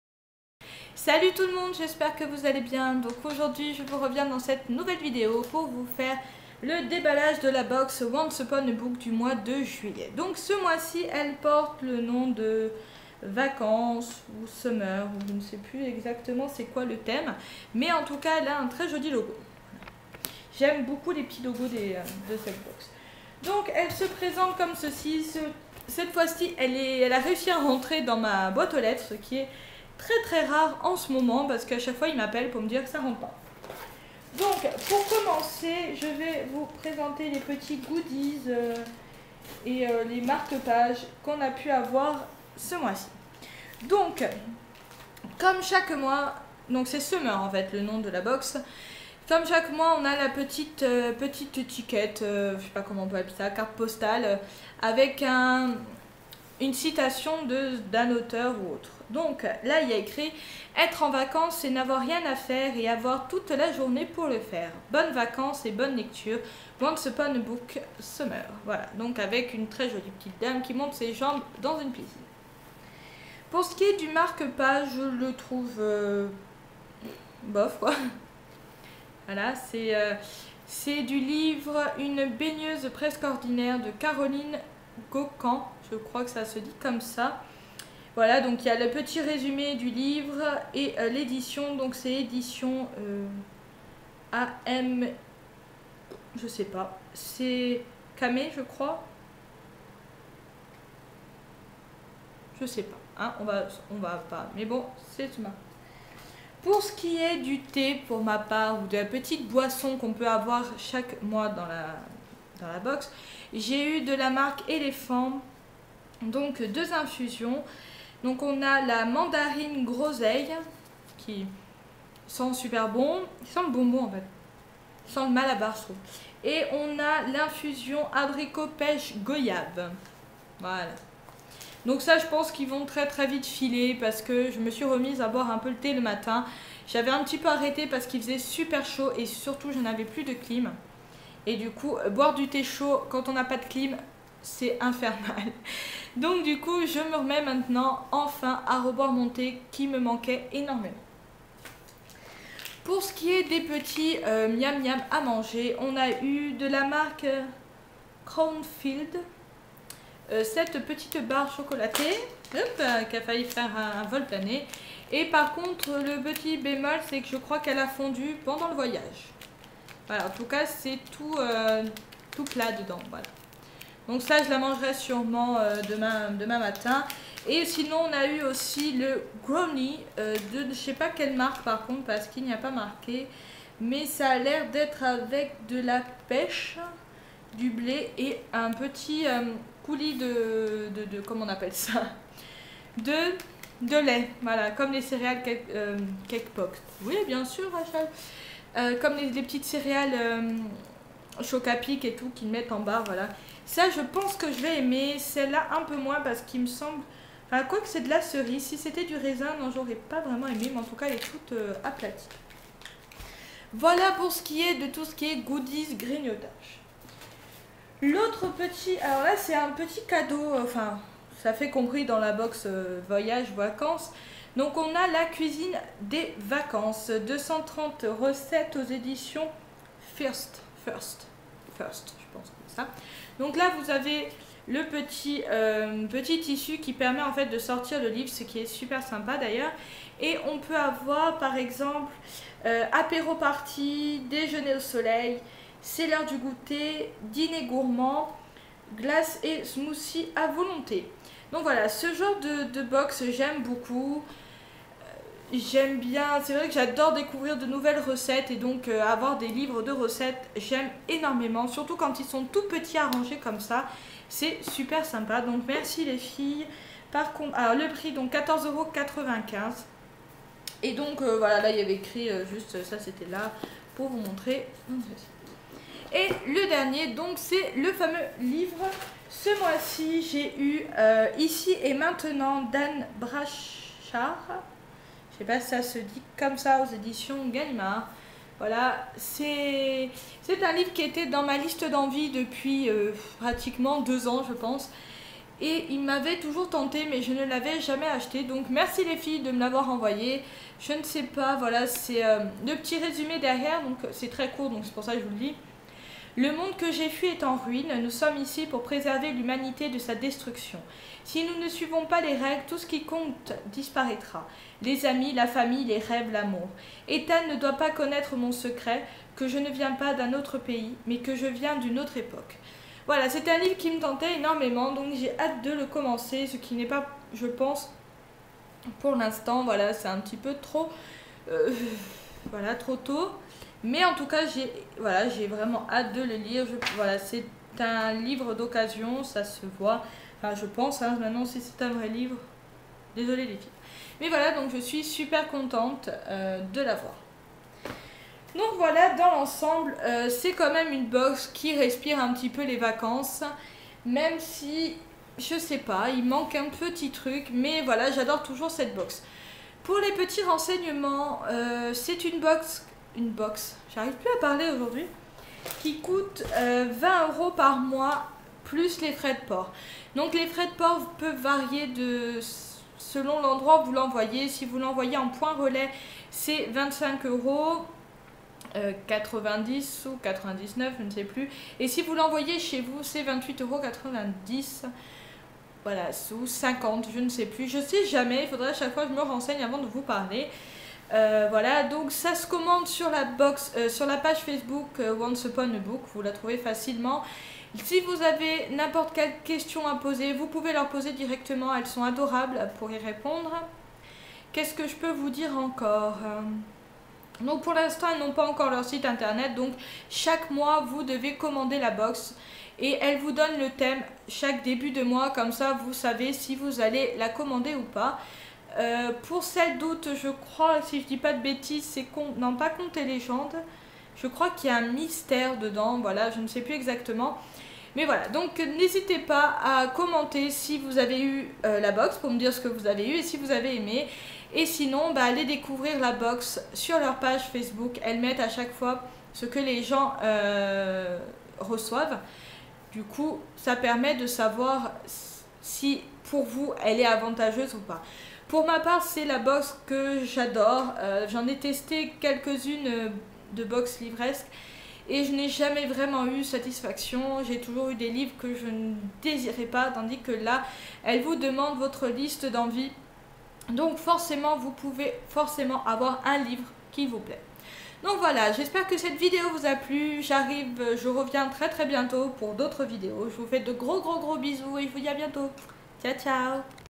Once Upon a book juillet
Hello mes petits abonnée
Aujourd'hui je vous présente le contenue de la box Once Upon a Book du mois de juillet. Elle a pour thème les vacances.